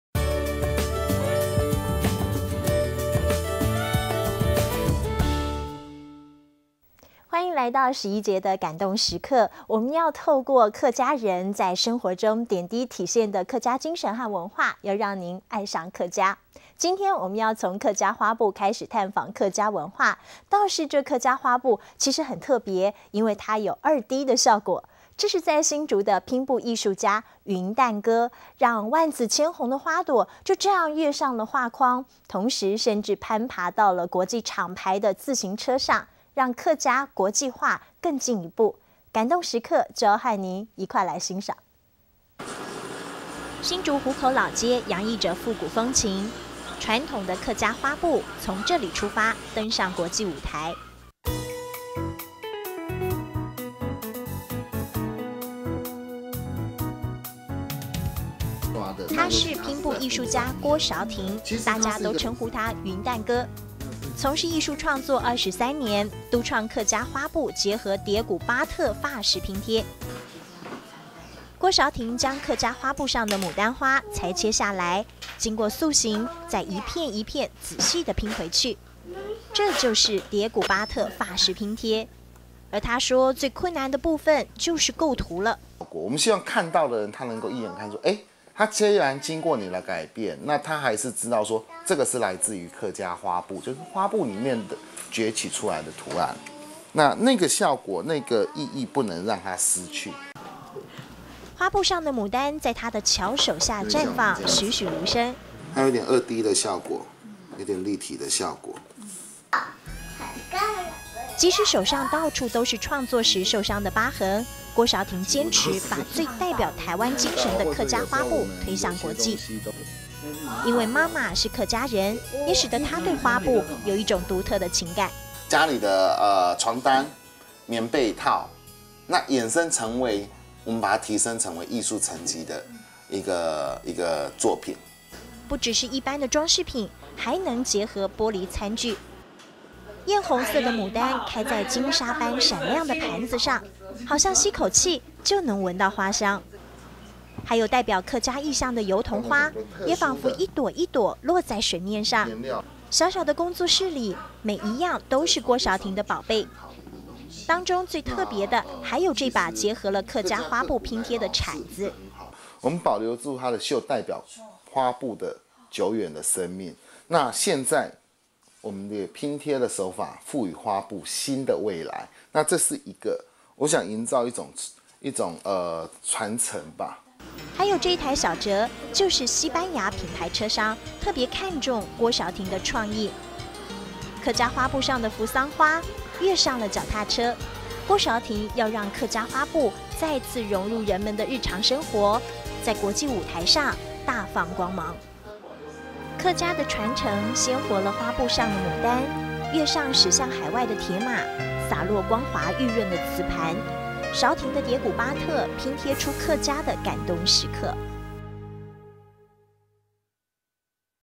蝶古巴特拼貼-展現客家之美
將台灣客家特色融合西方拼貼技術在國際發光，讓外國人看見台灣藝文之美，展現客家多元面貌。蝶古巴特拼貼專家郭玿廷與得力助手張雅婷，以立體拼貼花布，運用回收玻璃瓶與客家花布當作創作素材，製作出獨具台灣客家文化特色的玻璃立體拼貼作品，客家之美潛在藝術之中展現出來。